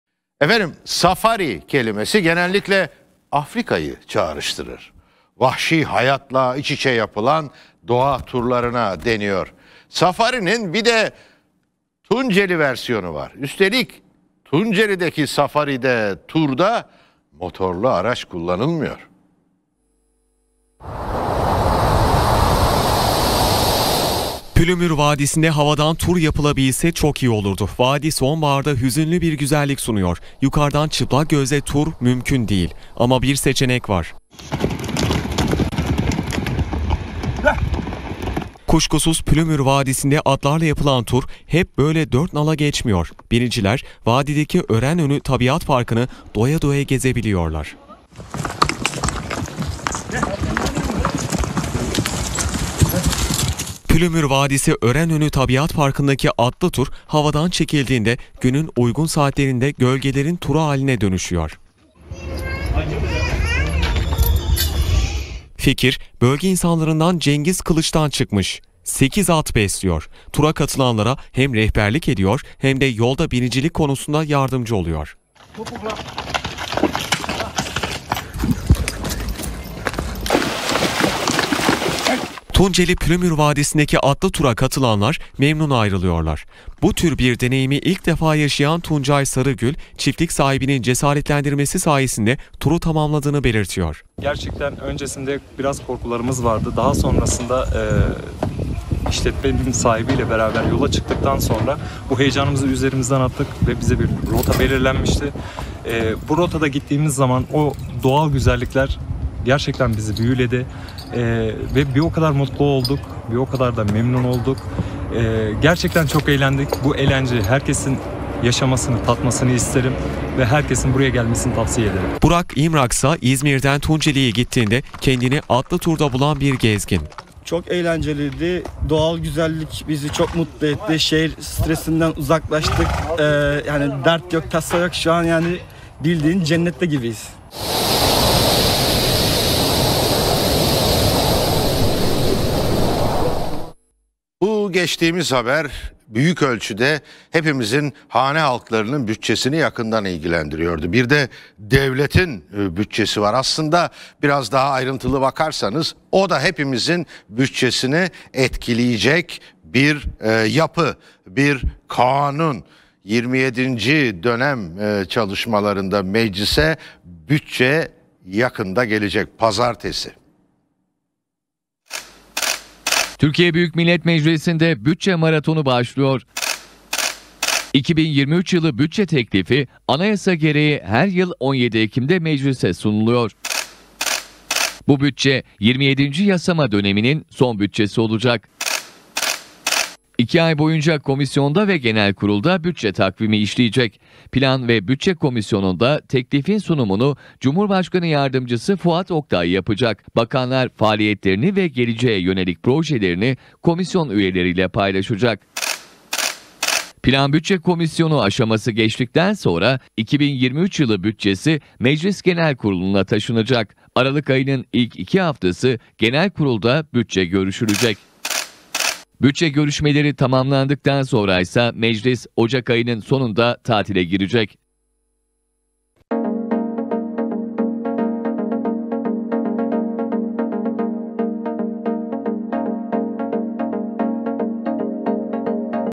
Bütçe 17 Ekim'de Meslis'e Sunulacak | Tv100 Haber
Bütçe maratonu başlıyor, 17 Ekim'de Meclis'e sunulacak.

TÜRKSAT 4A
Frekans: 12729
Sembol: 30000
Polarizasyon: V (Dikey)